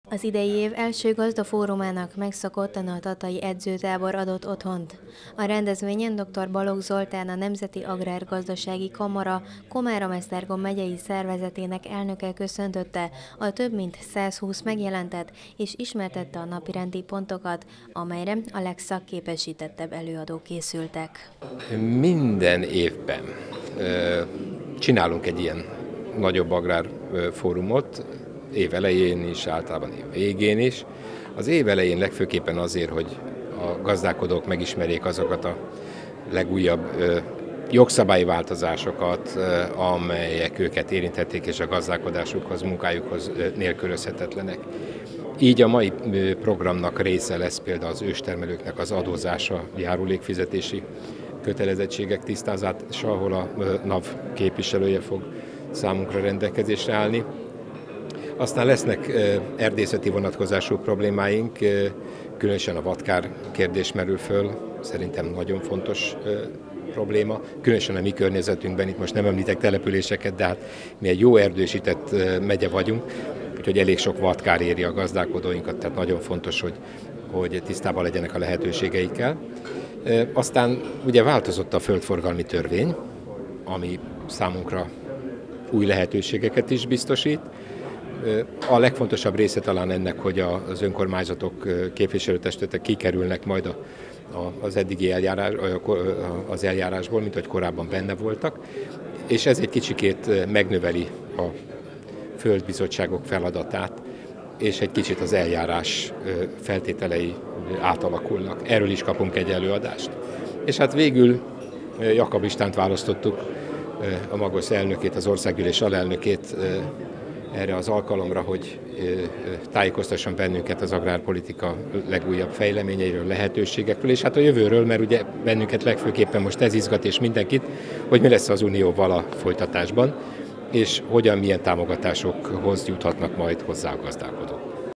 190221 Híradó 04 Gazdafórumot szerveztek az edzőtáborban
A magyar agrárium helyzetéről és időszerű kérdéseiről, az idei agrárpályázatokról, pályázati változásokról és lehetőségekről, valamint a Nemzeti Agrárgazdasági Kamara tavalyi eredményeiről és idei céljairól tájékoztatták Komárom-Esztergom megye gazdáit a szokásos év eleji fórumon, amelyet ezúttal is az edzőtáborban szerveztek meg.